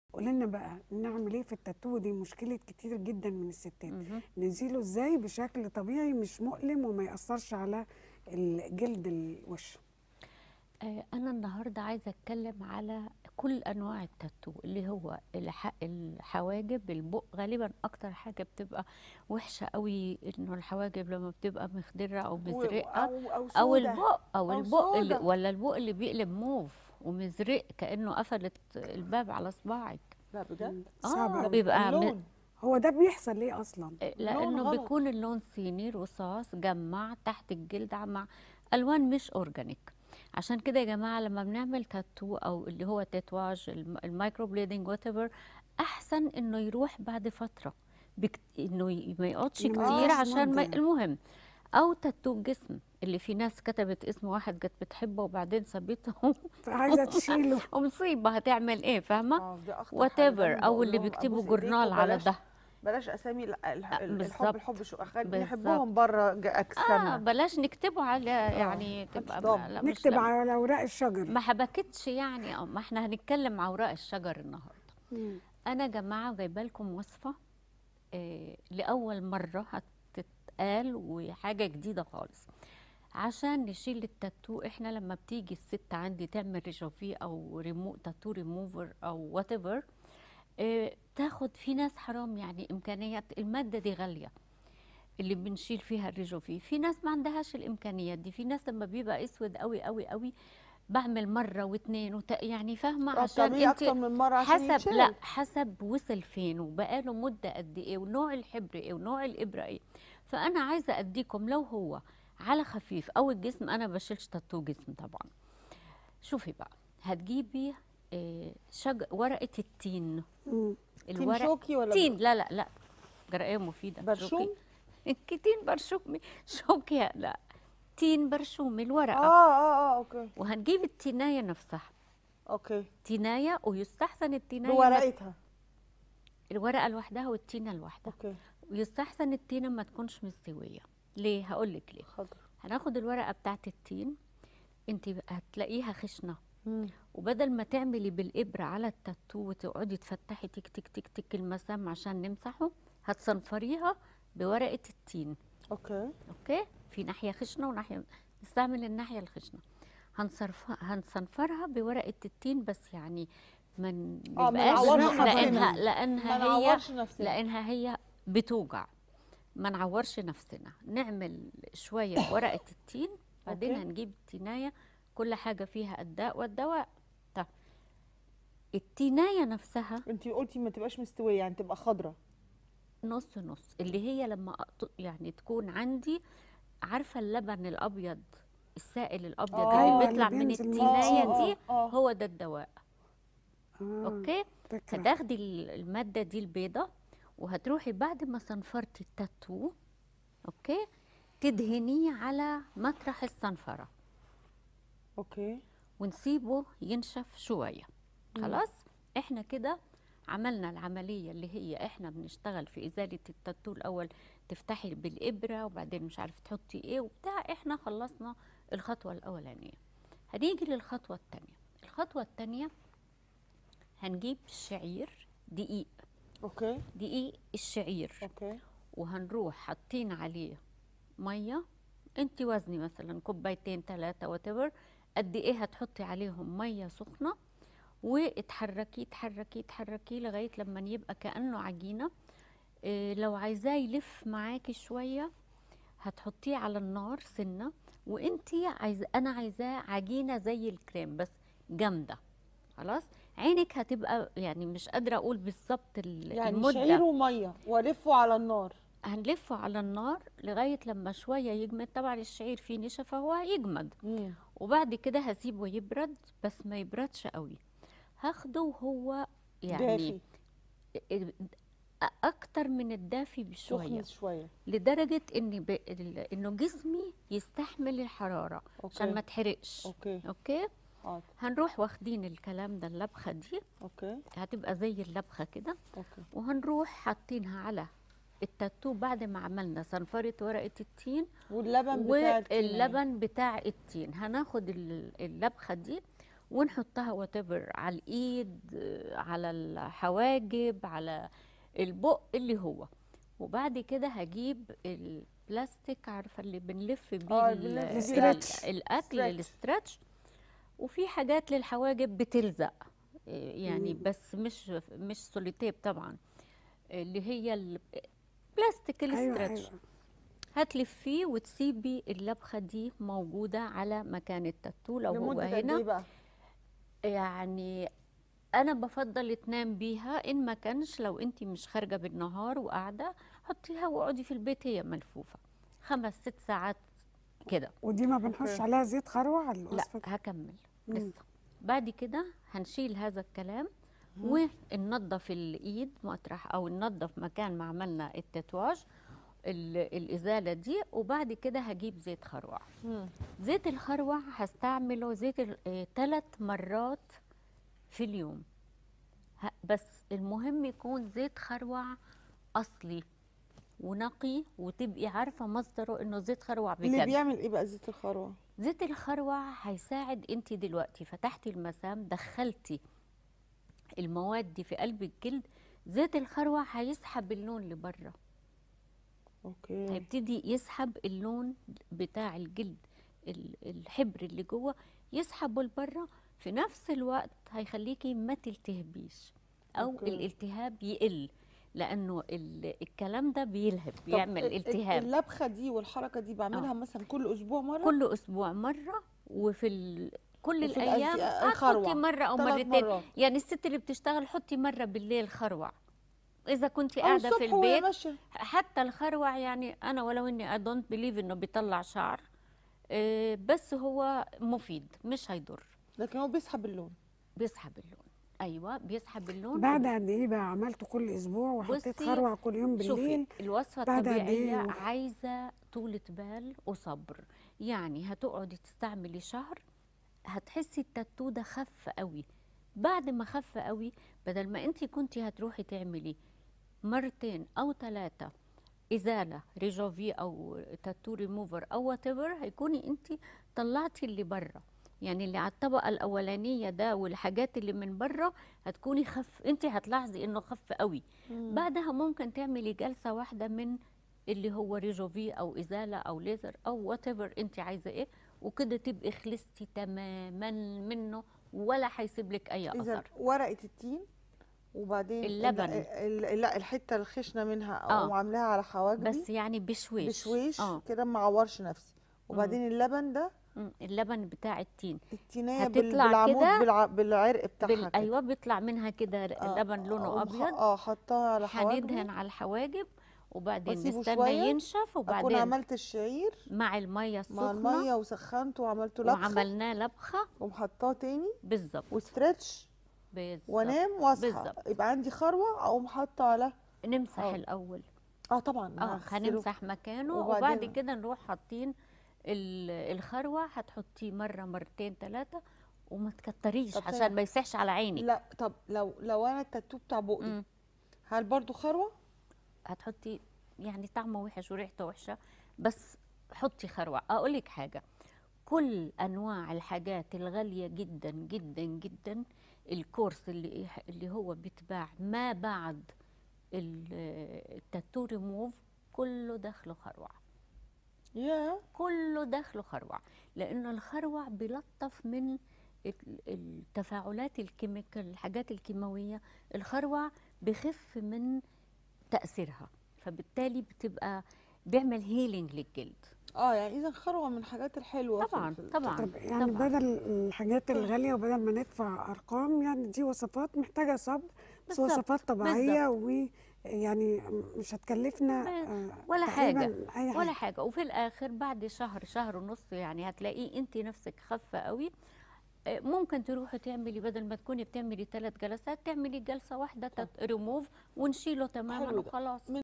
بدون ليزر ولا تكاليف وصفة سهلة وبسيطة هتعمليها في البيت لإزالة التاتو بشكل طبيعي ونهائي
الستات | بدون ليزر ولا تكاليف وصفة سهلة وبسيطة هتعمليها في البيت لإزالة التاتو بشكل طبيعي ونهائي